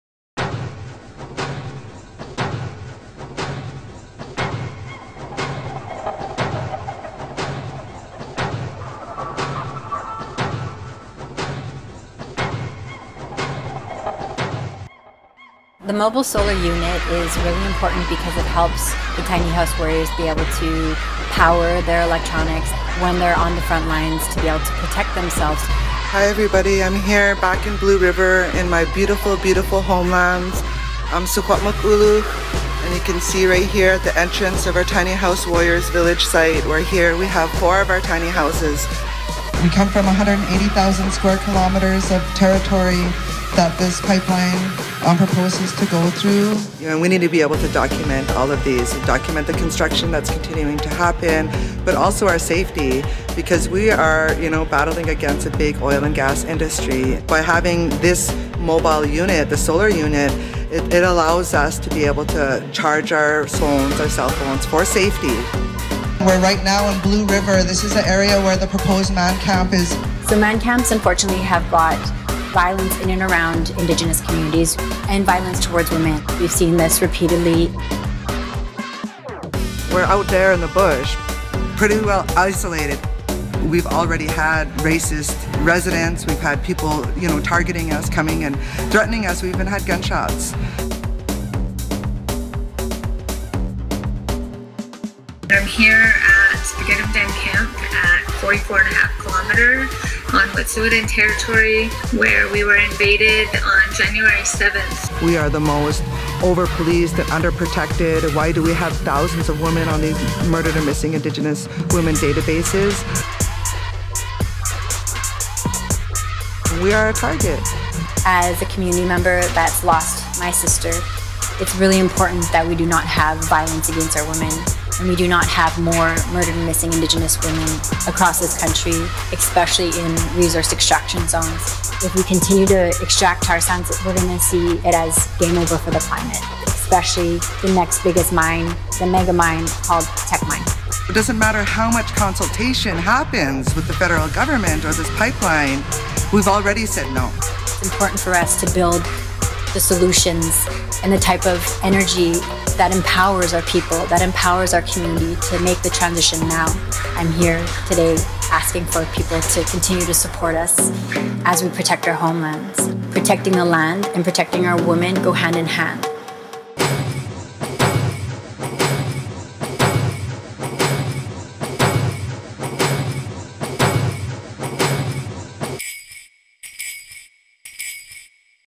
Sacred Earth Solar
"Protecting the land and protecting our women go hand in hand."

This mobile solar unit is helping to protect Indigenous Women on the Frontlines of Pipeline Resistance.

A video by - Melina Laboucan-Massimo
Edited by Tia Taurere-Clearsky of Whaea Productions